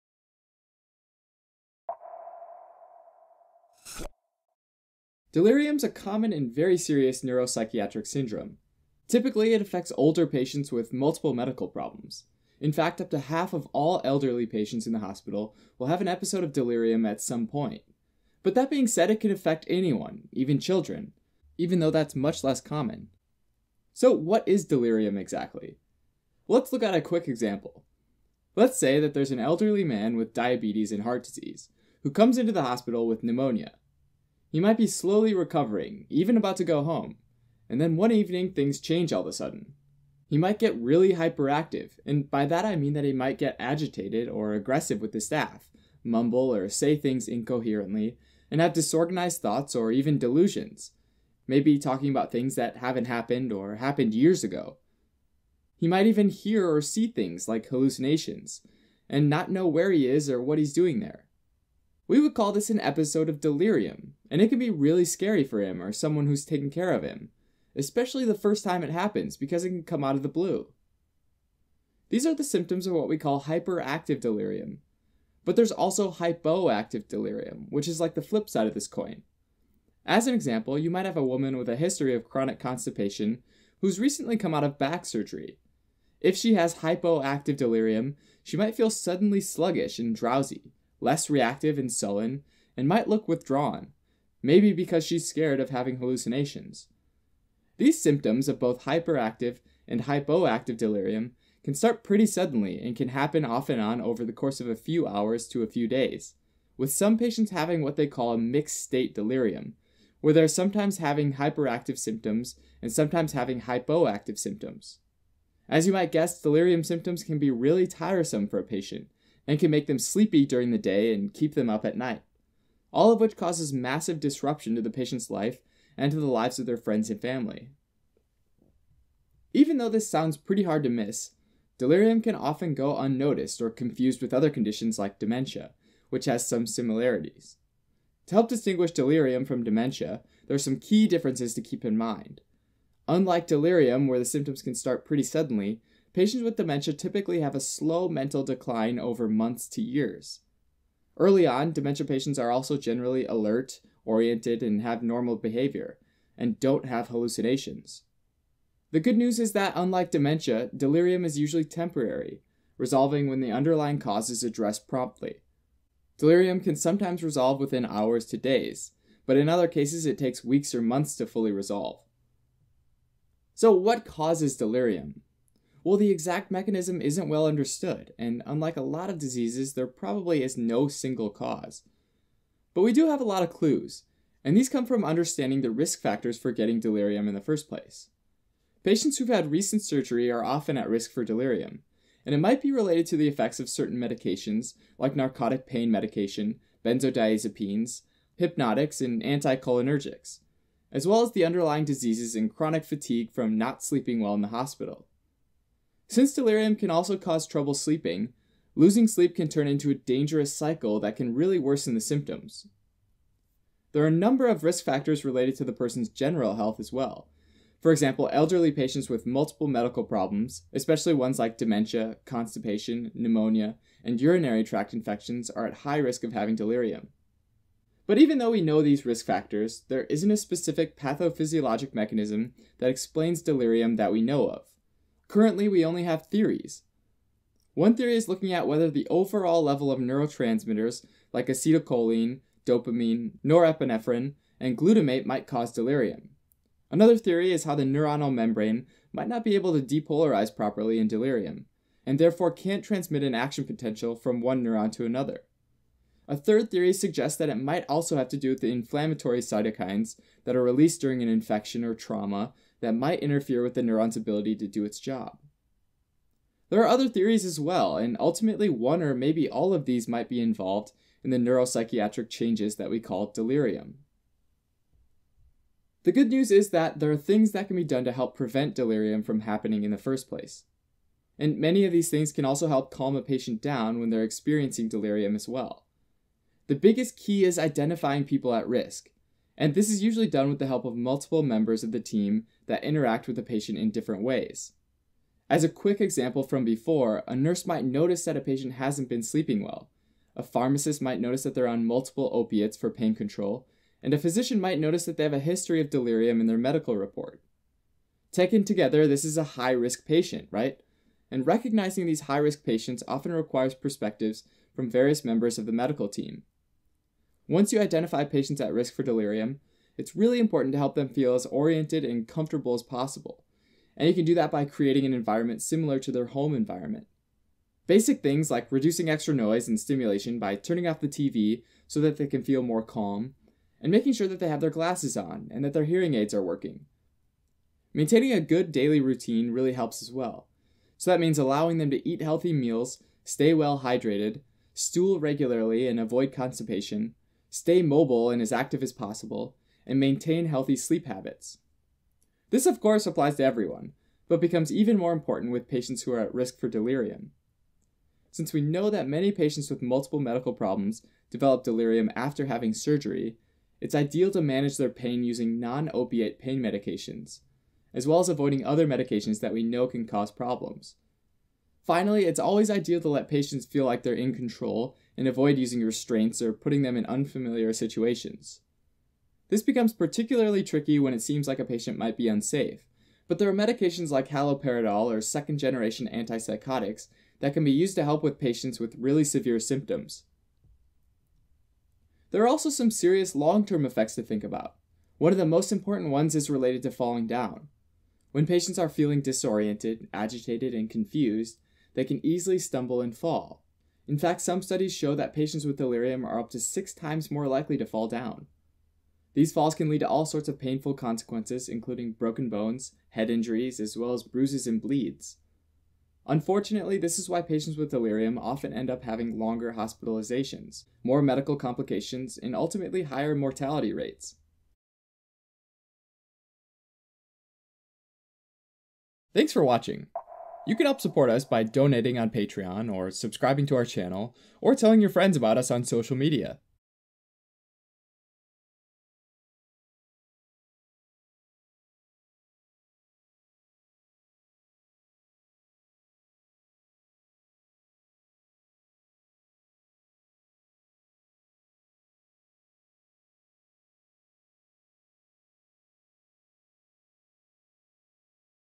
Delirium - causes, symptoms, diagnosis, treatment & pathology
What is delirium? Delirium is a sudden disturbance in mental abilities that can last for hours to days. Some patients experience hyperactive symptoms and some patients experience hypoactive symptoms, whereas some experience both.

This video describes the symptoms, pathophysiologic theories, and important risk factors.